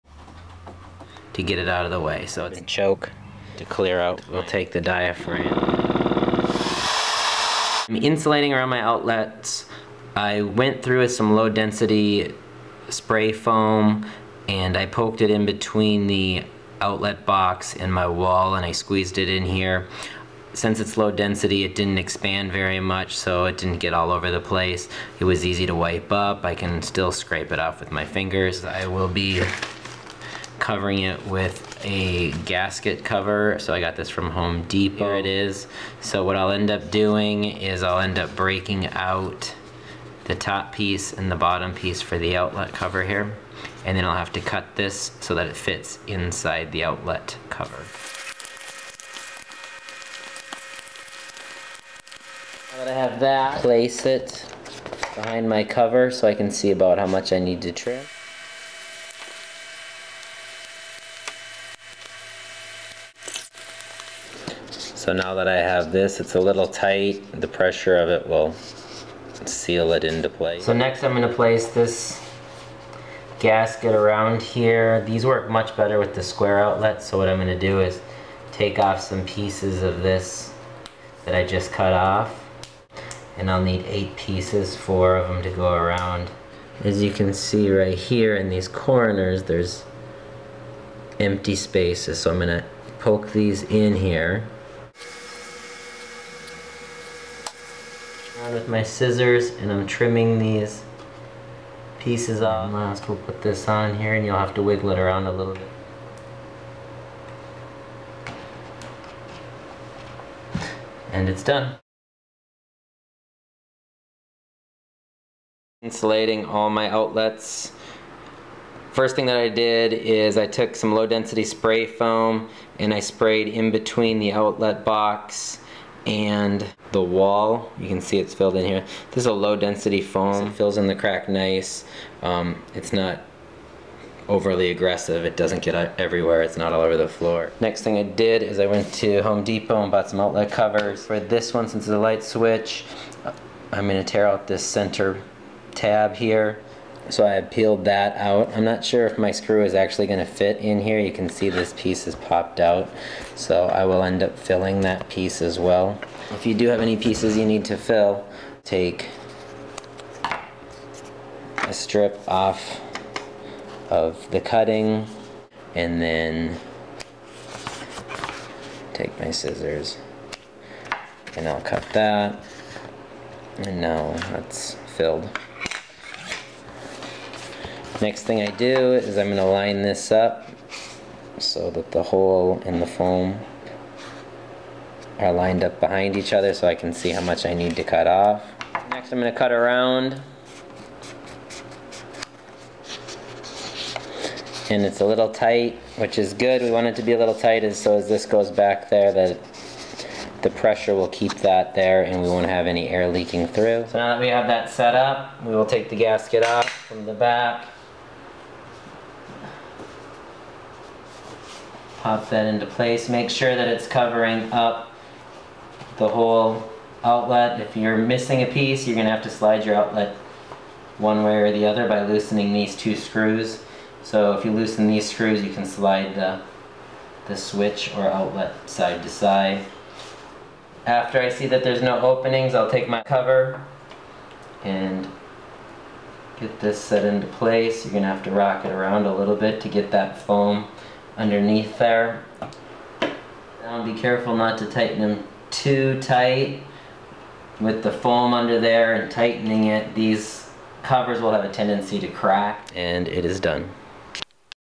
DIY INSULATE ELECTRIC OUTLETS LIGHT SWITCHES *** SPRAY FOAM GASKETS***
how do I INSULATE ELECTRICAL OUTLETS and LIGHT SWITCHES *** GASKETS and FOAM ***
Spending some time foaming around outlet boxes and placing foam gaskets around outlet boxes.  We had a number of outlets that were streaming cold air from the sides and through the box.  We could feel the draft coming through the box.  Since this has been done we have not noticed the cold air blowing into the room, so it has made a difference.  This was a simple and easy procedure.  I wouldn't be surprised if the first winter they paid for themselves.  Don't spray foam inside the outlet boxes as it is a fire hazard and TURN OFF electrical POWER to the outlets you are doing this too for safety reasons.  SHock/death HAZARD